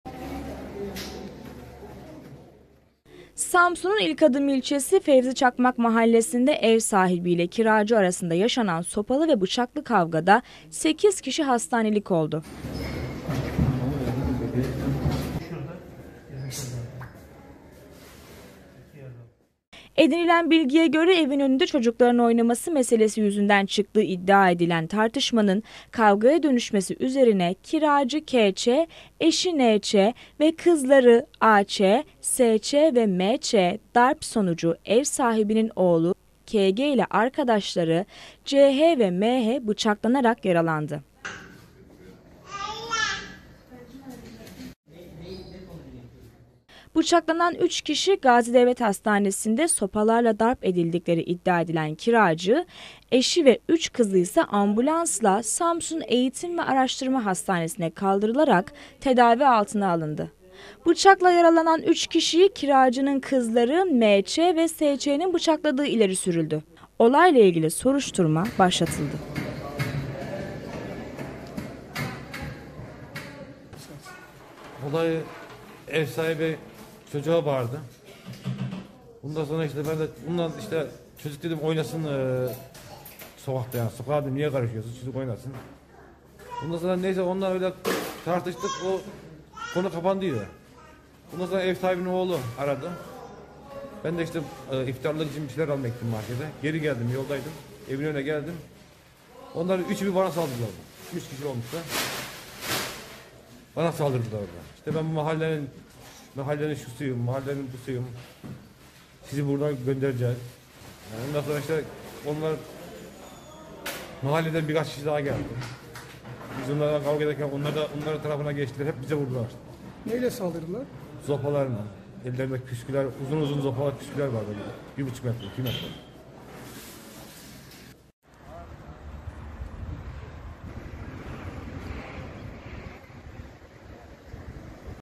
Samsun'da ev sahibi ile kiracı arasında sopalı ve bıçaklı kavga! 8 yaralı
Samsun Haber

Samsun'un İlkadım ilçesi Fevziçakmak Mahallesi'nde ev sahibi ile kiracı arasında yaşanan sopalı ve bıçaklı kavgada 8 kişi hastanelik oldu.